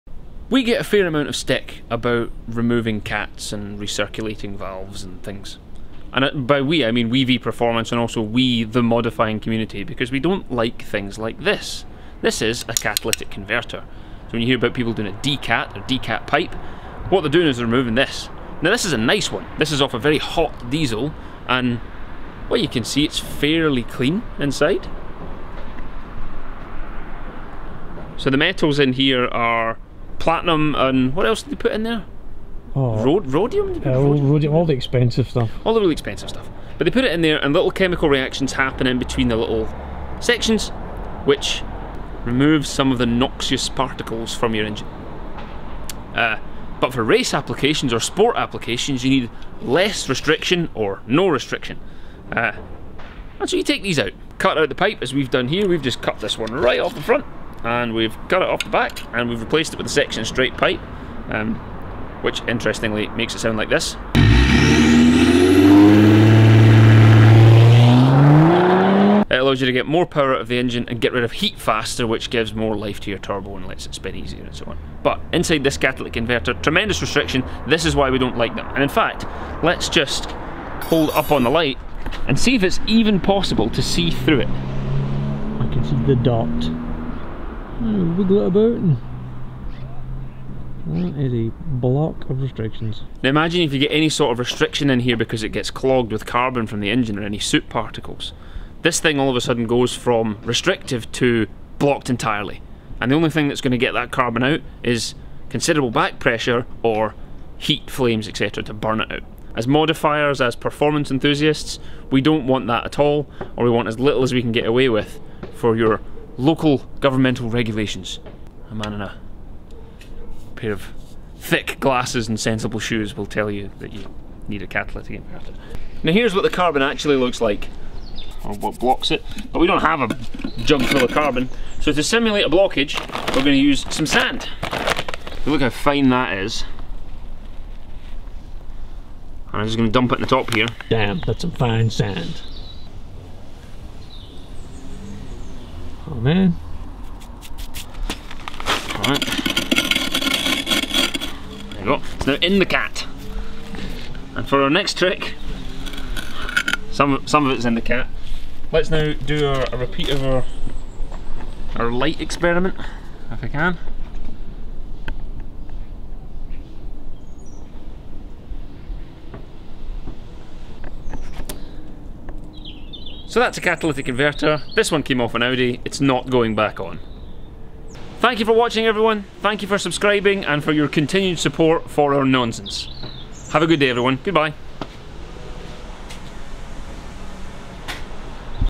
What's inside a catalytic converter
We kept the Audi's cat so we could show you what's inside.